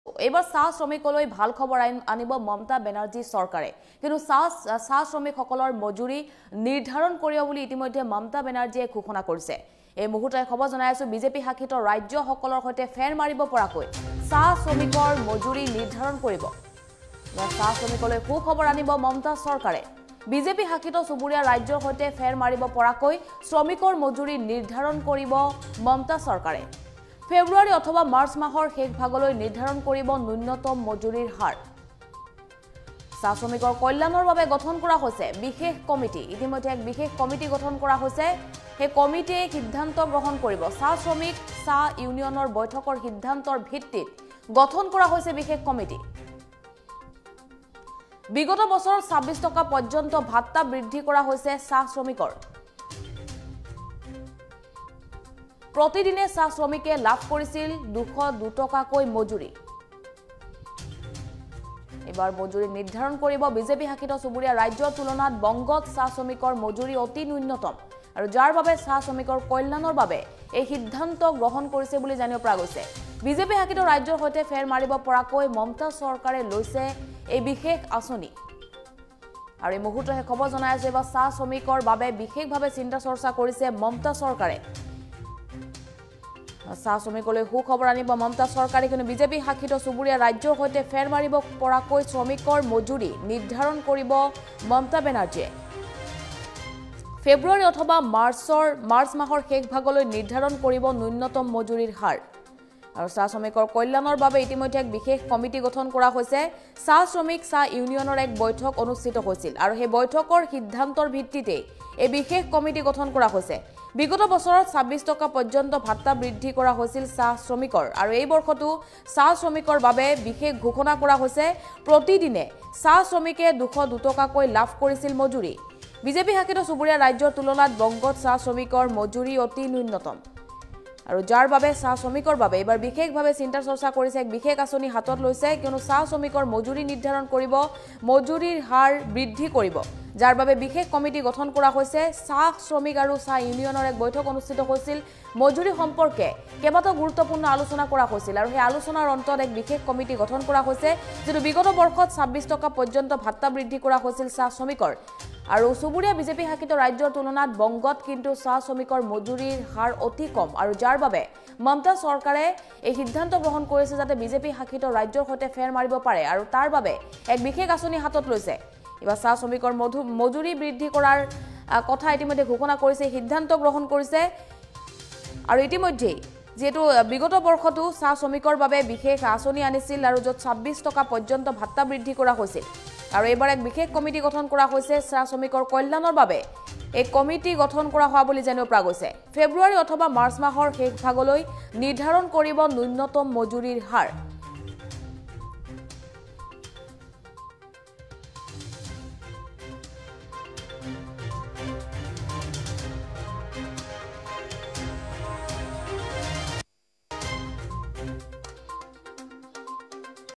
চাহ শ্ৰমিকলৈ সুখবৰ আনিব মমতাৰ চৰকাৰে
চাহ শ্ৰমিকলৈ সুখবৰ আনিব মমতাৰ চৰকাৰে। ফেব্ৰুৱাৰী অথবা মাৰ্চ মাহৰ শেষভাগলৈ নিৰ্ধাৰণ কৰিব নূ্ন্যতম মজুৰী হাৰ।